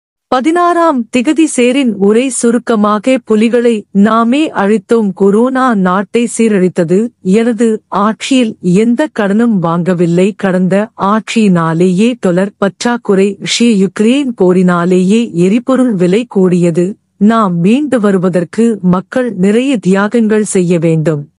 ஜனாதிபதி கோட்டாபய ராஜபக்ஷ அவர்கள் மக்களுக்கு வழங்கவுள்ள விசேட உரையின் சுருக்கம்
16ம் திகதி சேரின் உரை சுருக்கமாக 👌

*புலிகளை நாமே அழித்தோம்

*கொரோனா நாட்டை சீரழித்தது

*எனது ஆட்சியில் எந்தக் கடனும் வாங்கவில்லை

*கடந்த ஆட்சியினாலேயே டொலர் பற்றாக்குறை

*ரஷ்ய யுக்ரேன் போரினாலேயே எரிபொருள் விலை கூடியது

*நாம் மீண்டு வருவதற்கு மக்கள் நிறைய தியாகங்கள் செய்ய வேண்டும்